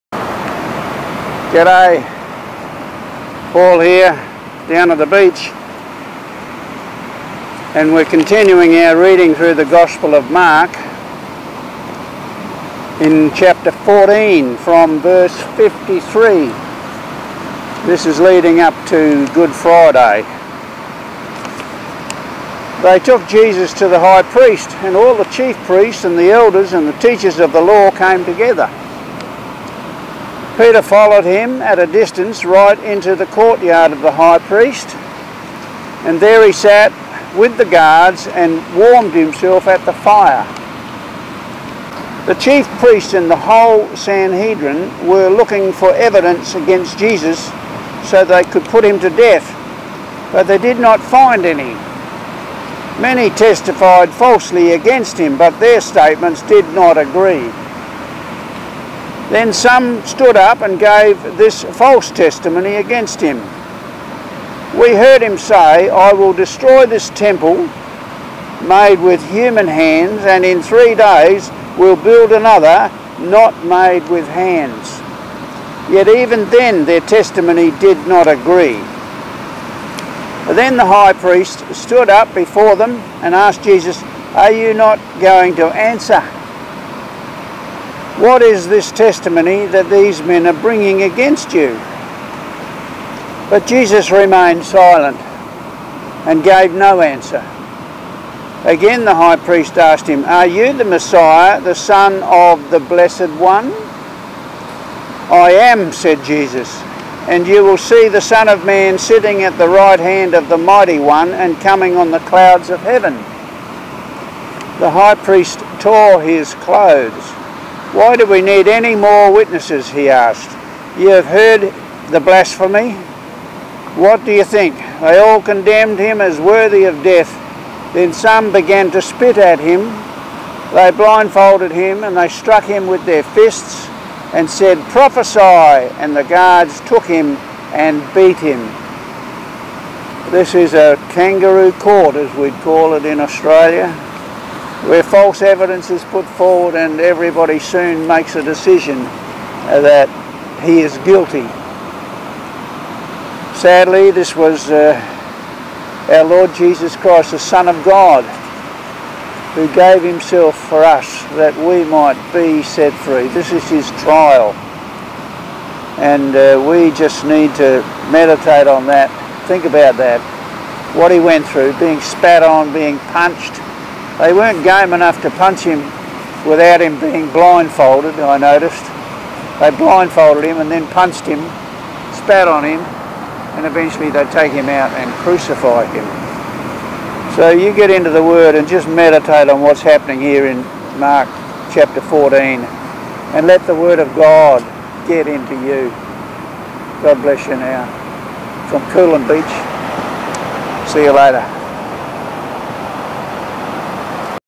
Kangaroo Court For Jesus Mark 14 F
Aussie Bible Study - Mark 14
Jesus Before the Sanhedrin
53 They took Jesus to the high priest, and all the chief priests, the elders and the teachers of the law came together. 54 Peter followed him at a distance, right into the courtyard of the high priest. There he sat with the guards and warmed himself at the fire.
55 The chief priests and the whole Sanhedrin were looking for evidence against Jesus so that they could put him to death, but they did not find any. 56 Many testified falsely against him, but their statements did not agree.
57 Then some stood up and gave this false testimony against him:
 58 “We heard him say, ‘I will destroy this temple made with human hands and in three days will build another, not made with hands.’”
59 Yet even then their testimony did not agree.
60 Then the high priest stood up before them and asked Jesus, “Are you not going to answer? What is this testimony that these men are bringing against you?”
61 But Jesus remained silent and gave no answer.
Again the high priest asked him, “Are you the Messiah, the Son of the Blessed One?”
62 “I am,” said Jesus. “And you will see the Son of Man sitting at the right hand of the Mighty One and coming on the clouds of heaven.”
63 The high priest tore his clothes. “Why do we need any more witnesses?” he asked. 64 “You have heard the blasphemy. What do you think?”
They all condemned him as worthy of death. 65 Then some began to spit at him; they blindfolded him, struck him with their fists, and said, “Prophesy!” And the guards took him and beat him.